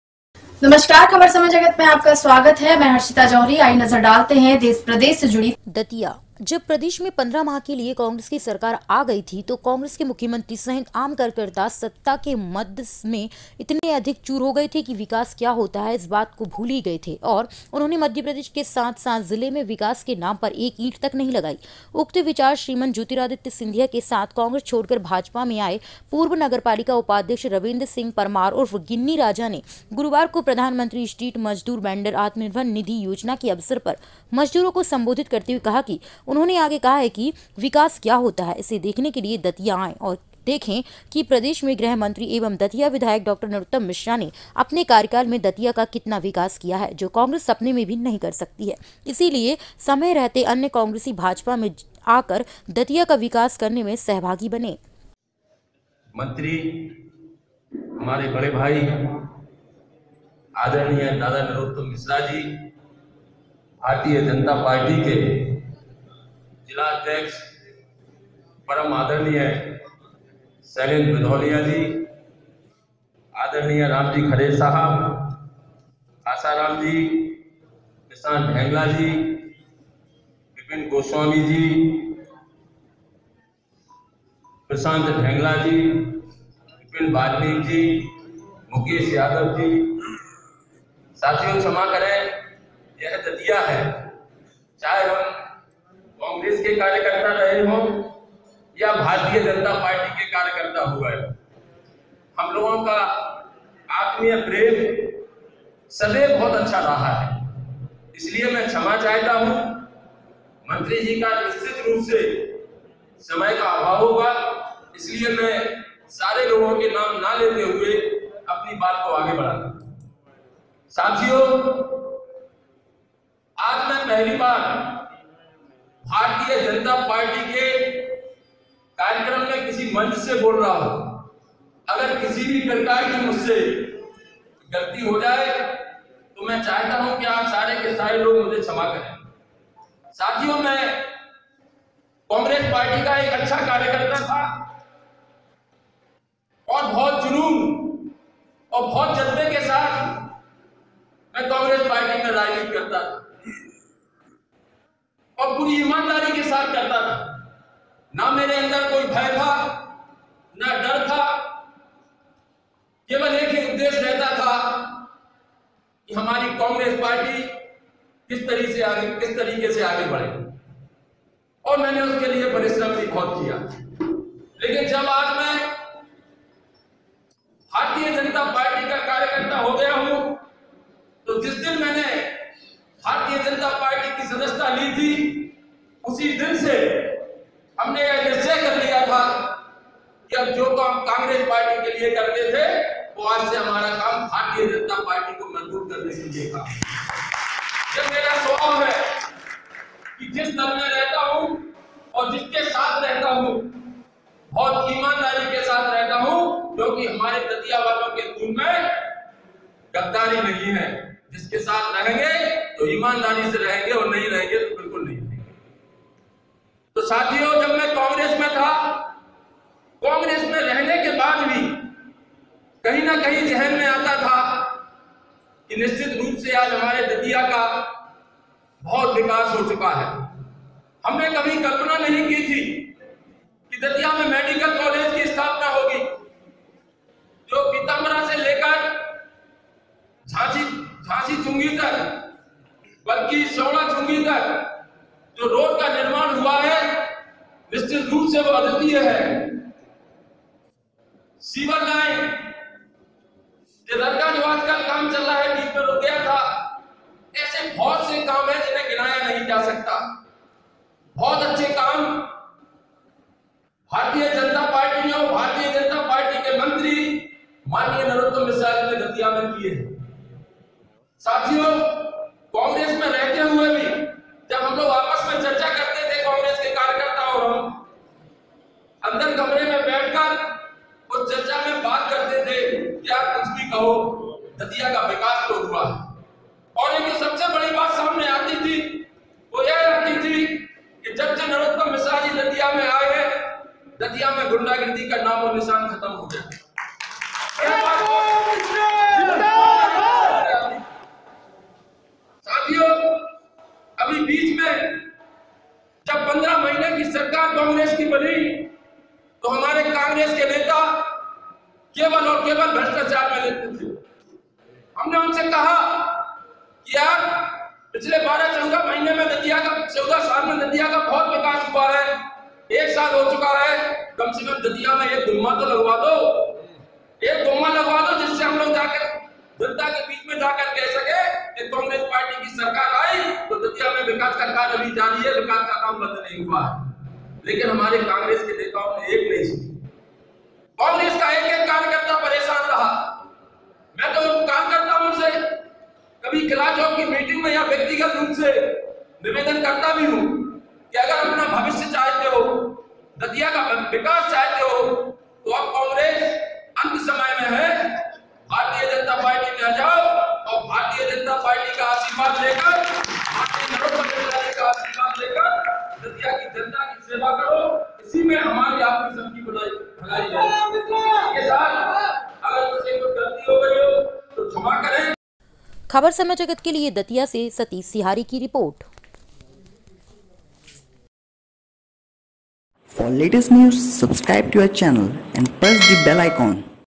दतिया - सत्ता के मद में चूर कांग्रेस ने विकास के नाम पर कोई कार्य नहीं किया
दतिया से सतीश सिहारे की रिपोर्ट।